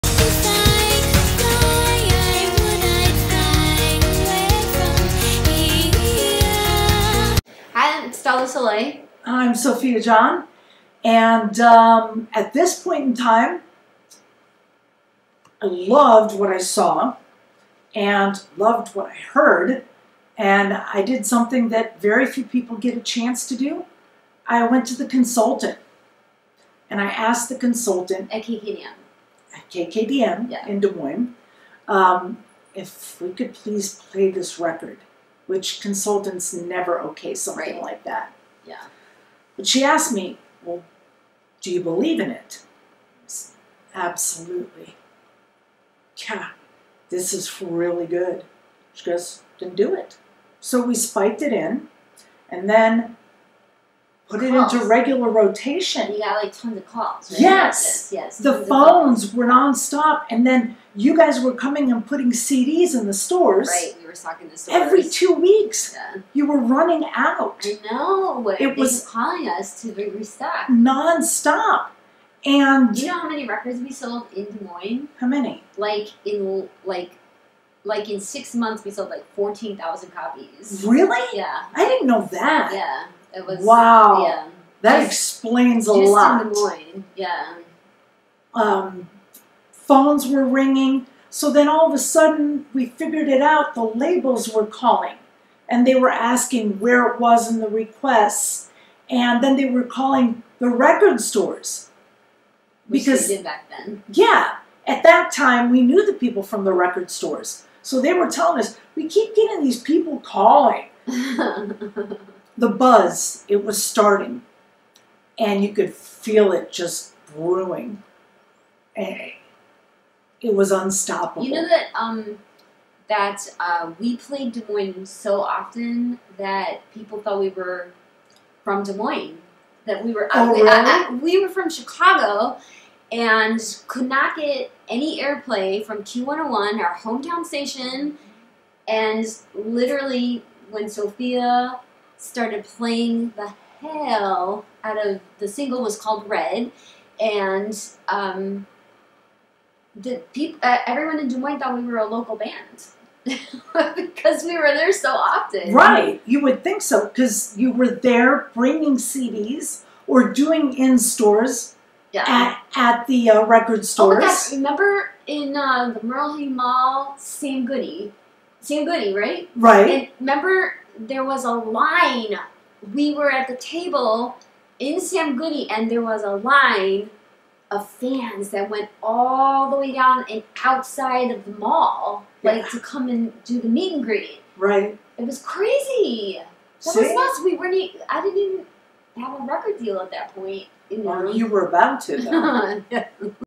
STELLA SOLEIL DOCUMENTARY: AN ALMOST POP PRINCESS EPISODE 17
This documentary spans 2 decades and talks about the rise and fall of a a popstar.  All the experiences and stories that go along with it.  And how to discover the meaning of life.  Enjoy! Stella